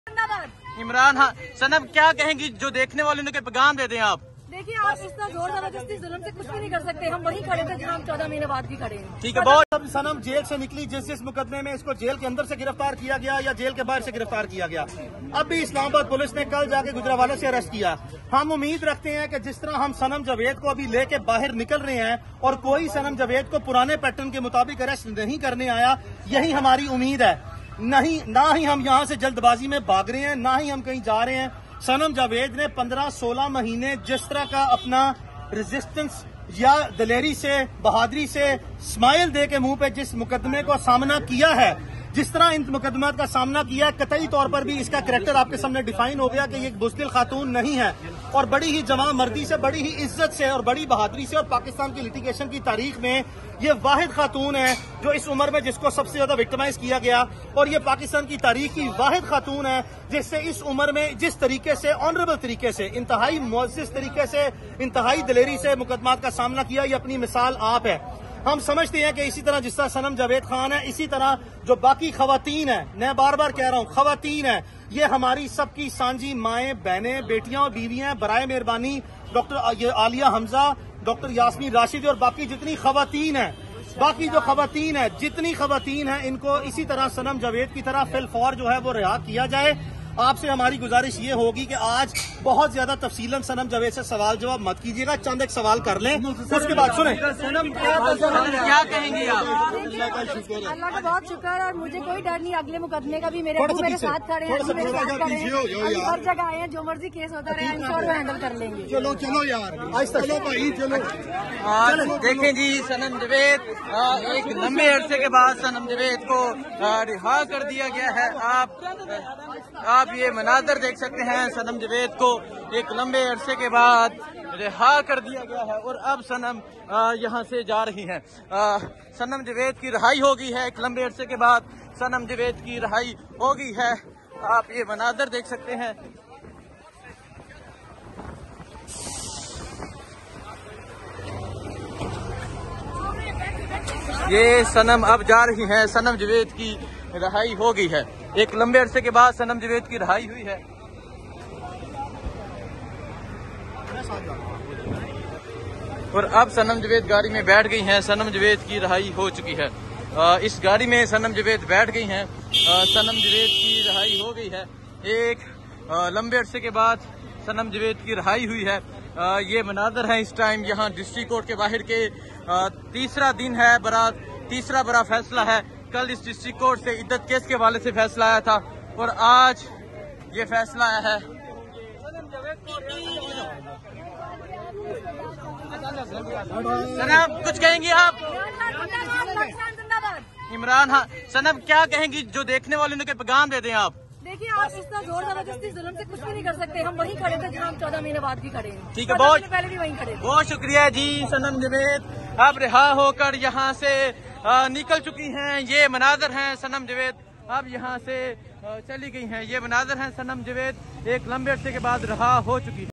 PTI's Sanam Javed Discharged from FIA Case | Raises Slogan "Imran Khan Zindabad"
بریکنگ نیوز : پی ٹی آئی کی   صنم جاوید رہا ۔۔اسلام آباد کی عدالت نے ایف آئی اے کے مقدمے سے ڈسچارج کردیا ۔۔ رہا ہوتے ہی صنم جاوید نے عمران خان  زندہ باد کا نعرہ لگایا۔۔دیکھیں پہلی گفتگو

🔴 Breaking News: PTI's Sanam Javed Discharged from FIA Case | Raises Slogan "Imran Khan Zindabad" Upon Release | First Conversation 🎥